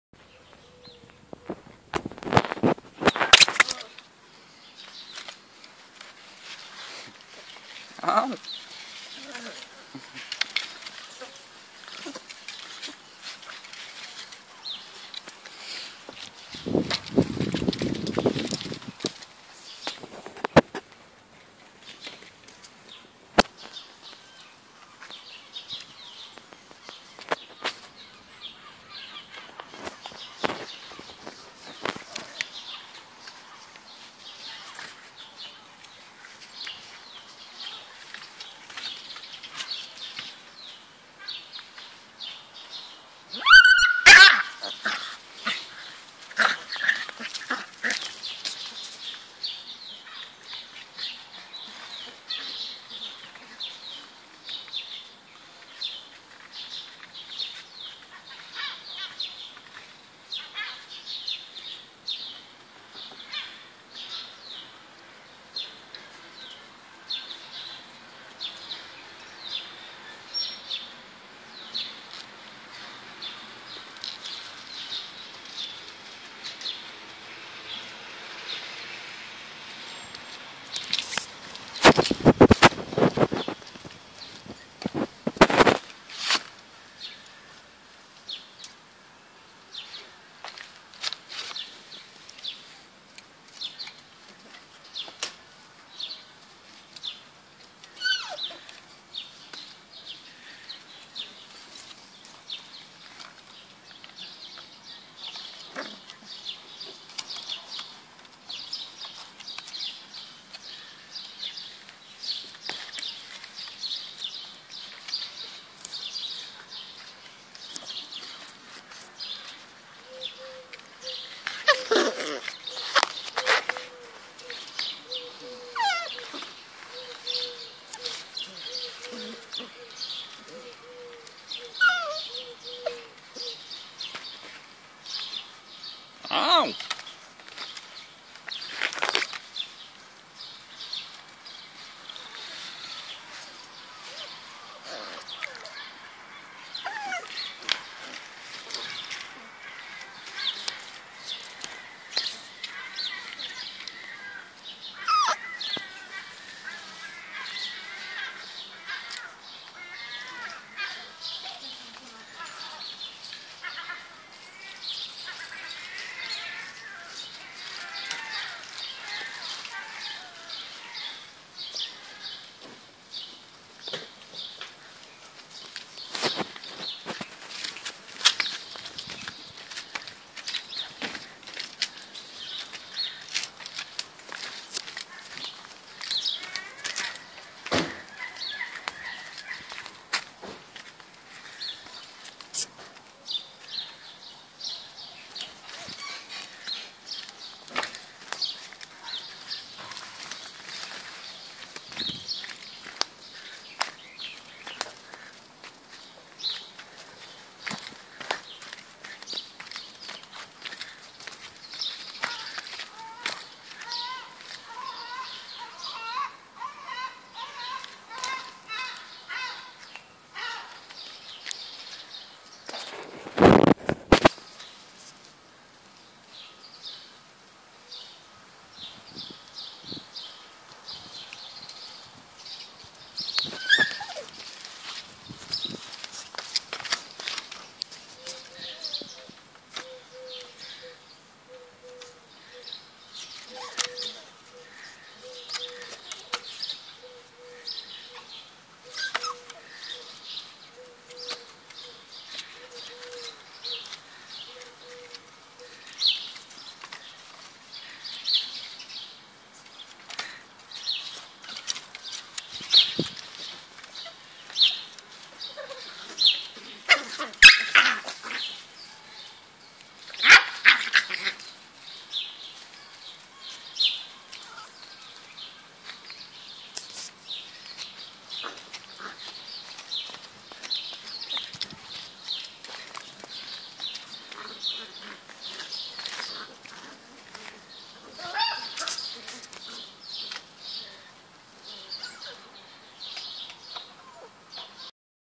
Eight 7 w o beagle puppies playing
very cute beagle puppies playing